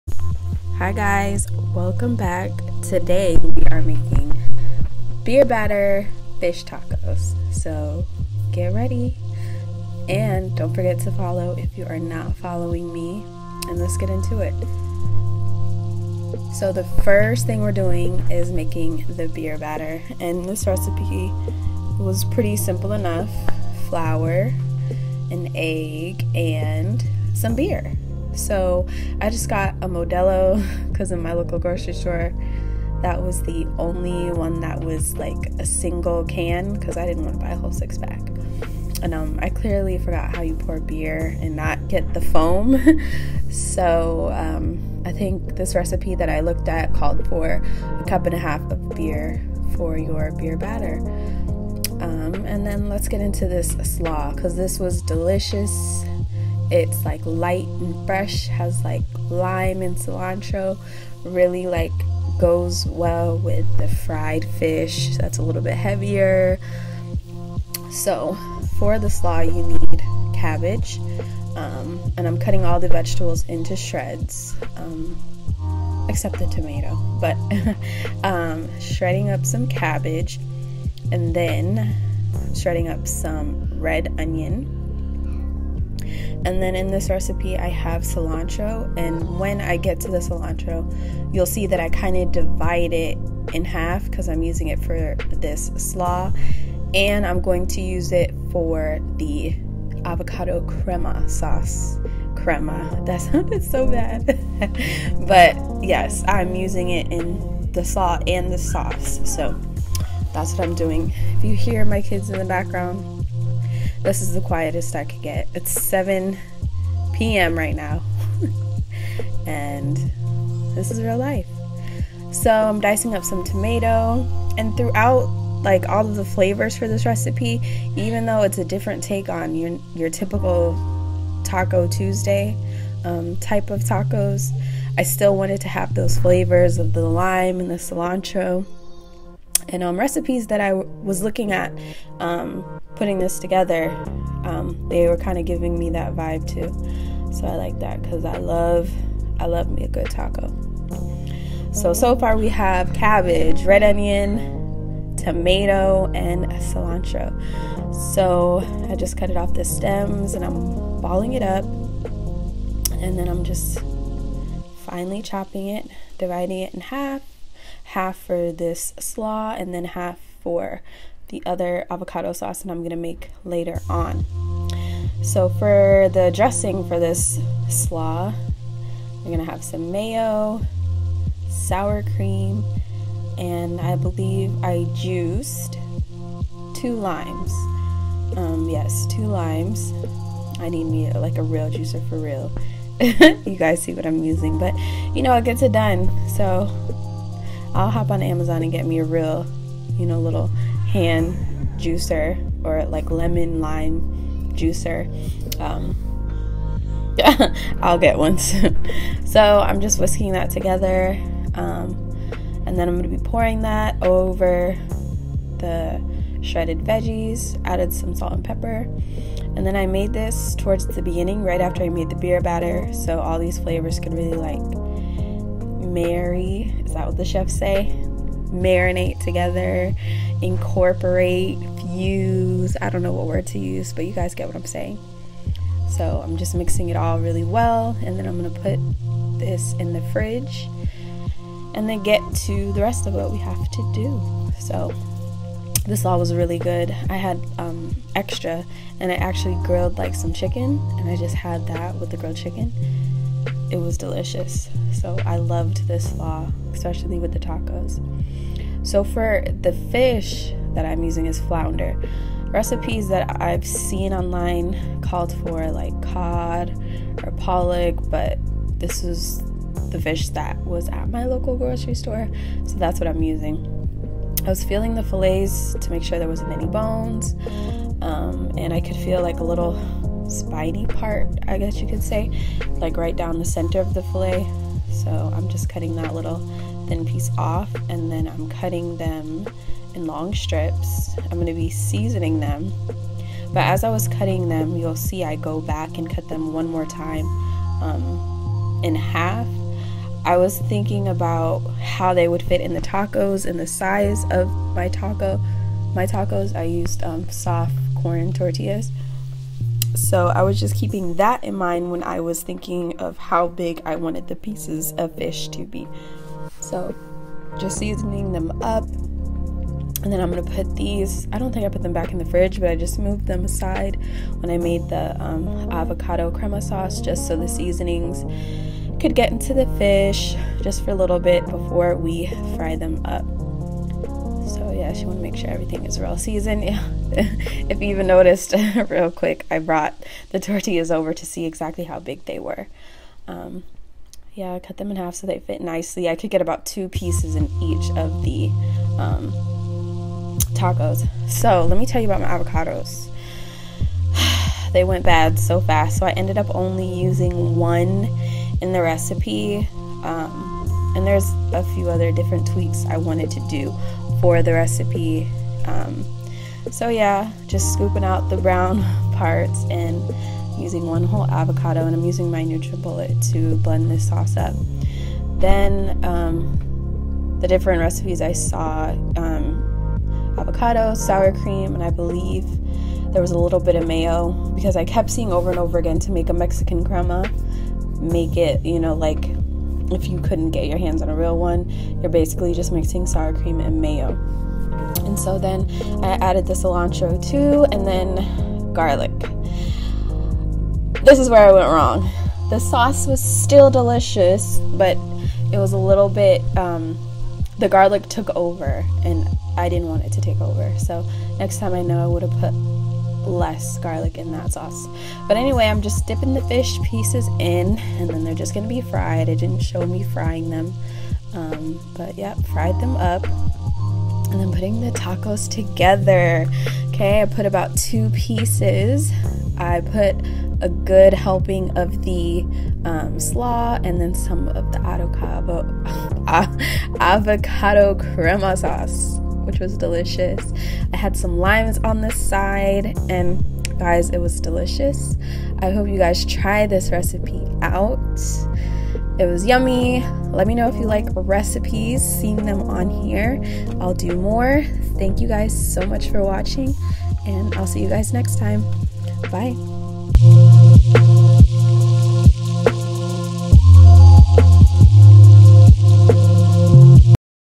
Beer Batter Fish Tacos |What part was a FAIL? | Nina Celeste
I wanted to change up taco night and I think I did great with these beer batter fish tacos. The beer batter was only 3 ingredients and the slaw and the avocado crema sauce are optional but delicious all together. I hope you try out this beer batter fish taco recipe and let me know what you think.

Go read my blog ninaceleste.com to learn more about this video and the products mentioned

FTC: Linked products listed above are ones that I love and maybe monetizable links